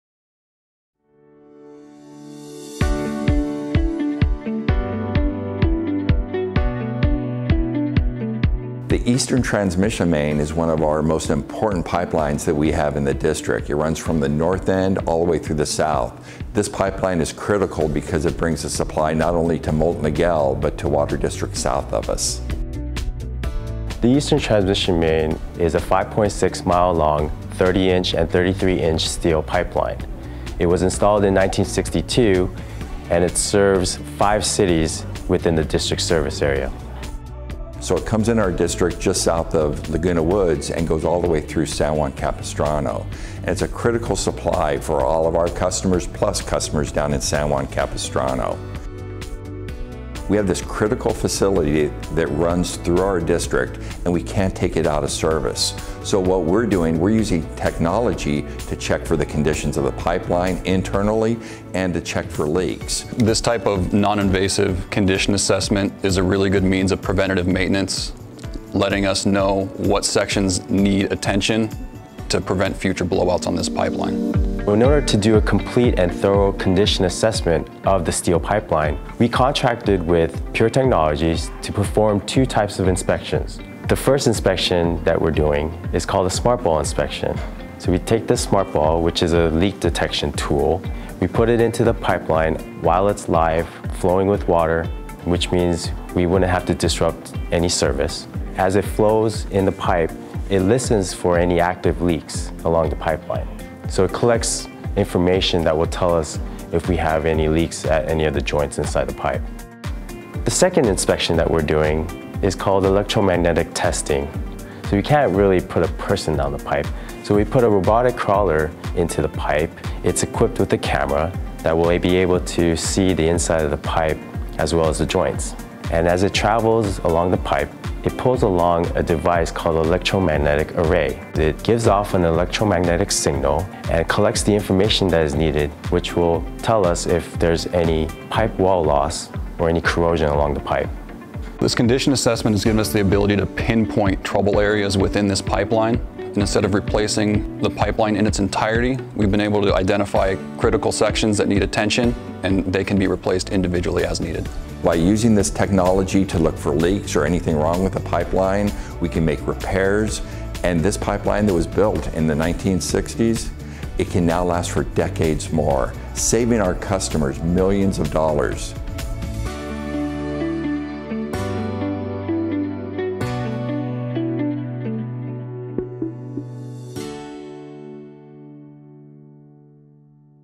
INNOVATIVE TECHNOLOGY: ENABLING SMARTER DECISION MAKING
Moulton Niguel is utilizing innovative technology to assess the condition of one of the most critical water transmission pipelines in the District. The 60-year-old Eastern Transmission Main is a 5.6-mile-long buried steel pipeline, starting in the City of Laguna Woods and traversing through Cities of Laguna Hills, Mission Viejo, Laguna Niguel, and San Juan Capistrano.

The  inspections provide data to pin-point specific areas within the pipeline that may require maintenance or rehabilitation. This allows the District to prioritize activities to maximize the life of this critical infrastructure and
ensure continued reliable service while also minimizing cost. Without this innovative technology, the District would have to rely on addressing leaks and pipe breaks as they occur or replace the entire pipeline. This project
is projected to save our customers millions of dollars over the life of the pipeline.